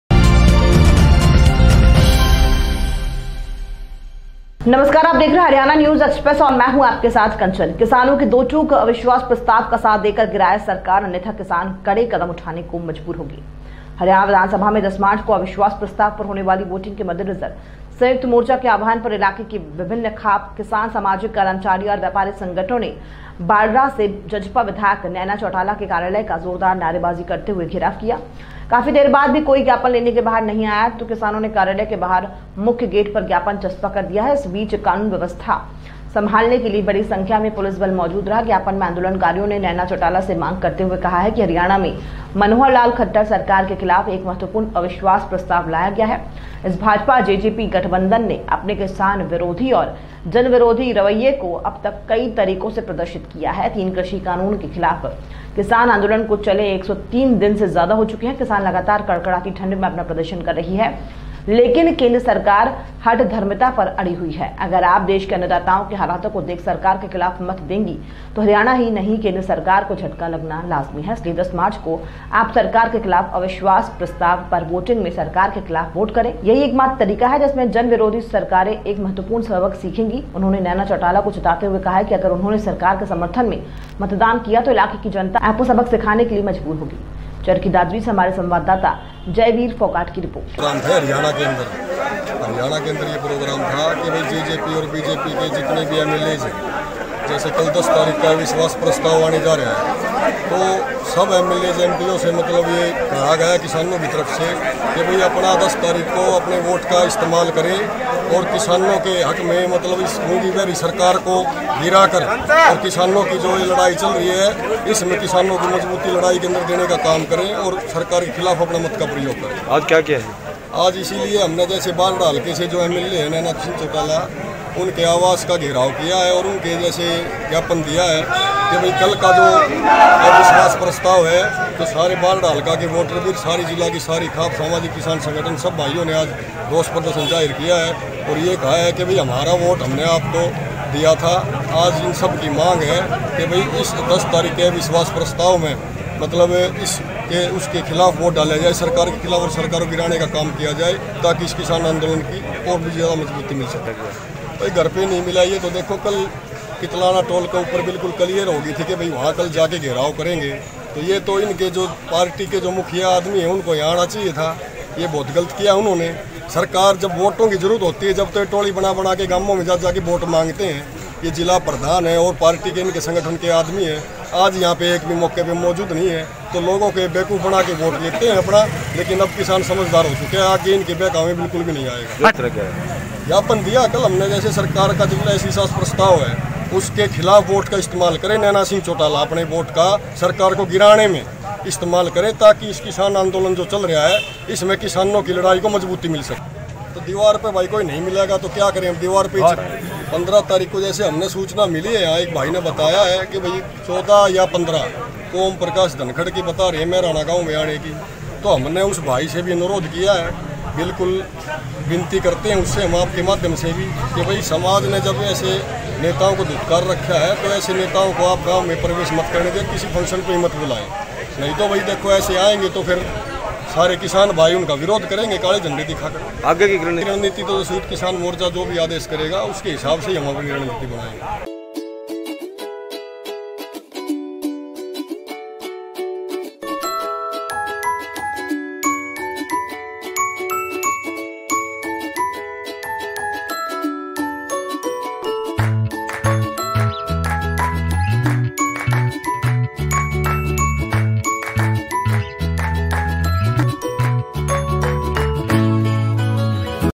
चरखी दादरी : किसानों ने विधायक नैना चौटाला के कार्यालय का घेराव कर गेट के बाहर चस्पा दिया ज्ञापन
हरियाणा विधानसभा में 10 मार्च को अविश्वास प्रस्ताव पर होने वाली वोटिंग के मद्देनजर संयुक्त मोर्चा के आह्वान पर इलाके की विभिन्न खाप, किसान, सामाजिक, कर्मचारी और व्यापारी संगठनों ने बाढड़ा से जजपा विधायक नैना चौटाला के कार्यालय का जोरदार नारेबाजी करते हुए घेराव किया। काफी देर के बाद भी कोई ज्ञापन लेने के बाहर नहीं आया तो किसानों ने कार्यालय के बाहर मुख्य गेट पर ज्ञापन चस्पा कर दिया। इस बीच कानून व्यवस्था संभालने के लिए बड़ी संख्या में पुलिस बल मौजूद रहा। ज्ञापन में आंदोलनकारियों ने नैना चौटाला से मांग करते हुए कहा कि हरियाणा में मनोहर लाल खट्टर सरकार के खिलाफ एक महत्वपूर्ण अविश्वास प्रस्ताव लाया गया है। इस भाजपा-जेजेपी गठबंधन ने अपने किसान विरोधी और जन विरोधी रवैये को अब तक कई तरीकों से प्रदर्शित किया है। तीन कृषि कानून के खिलाफ किसान आंदोलन को चले 103 दिन से ज्यादा हो चुके हैं किसान लगातार कड़कड़ाती ठंड में अपना प्रदर्शन कर रहे हैं। लेकिन केन्द्र सरकार हठधर्मिता पर अड़ी हुई है। अगर आप देश के अन्नदाताओं के हालातों को देख सरकार के खिलाफ मत देंगी तो हरियाणा ही नहीं केंद्र सरकार को झटका लगना लाजमी है। इसलिए 10 मार्च को आप सरकार के खिलाफ अविश्वास प्रस्ताव पर वोटिंग में सरकार के खिलाफ वोट करें। यही एकमात्र तरीका है जिसमें जन-विरोधी सरकारें एक महत्वपूर्ण सबक सीखेगी। उन्होंने नैना चौटाला को चेताते हुए कहा कि अगर उन्होंने सरकार के समर्थन में मतदान किया तो इलाके की जनता आपको सबक सिखाने के लिए मजबूर होगी।